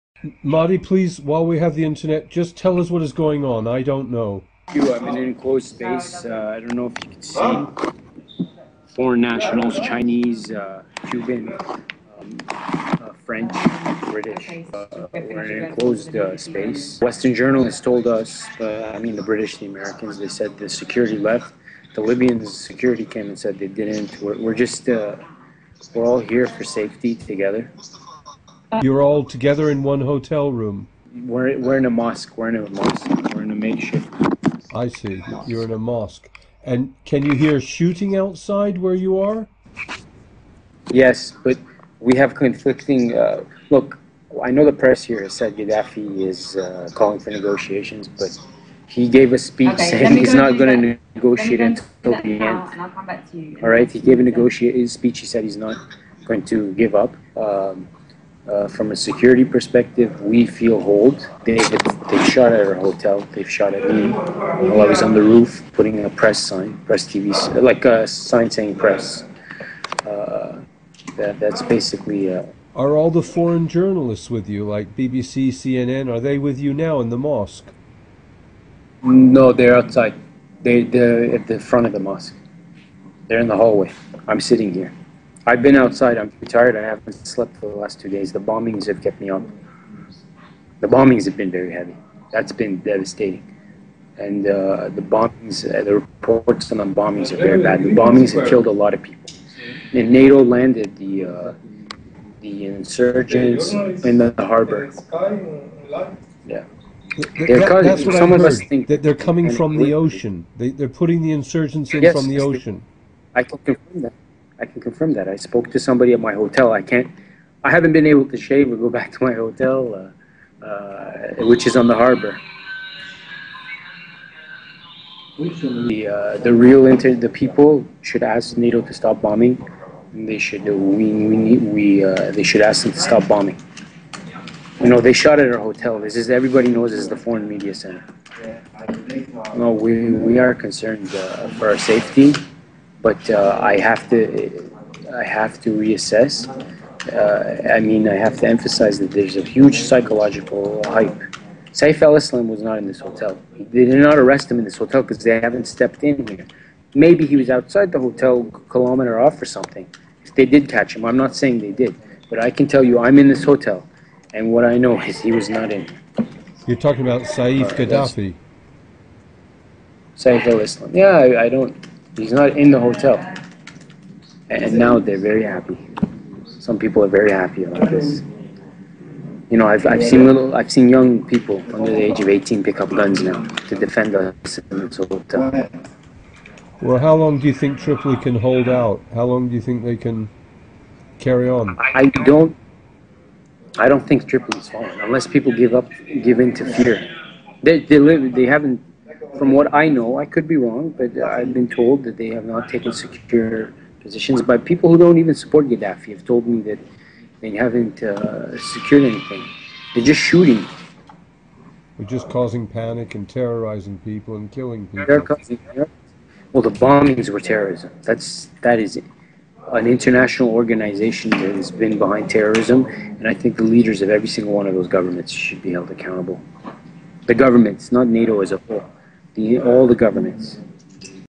NATO haven't secured anything they're just shooting - TRIPOLI
Holed up in a Mosque - after days of Bombing - with other foreigners - for their safety - his life is in danger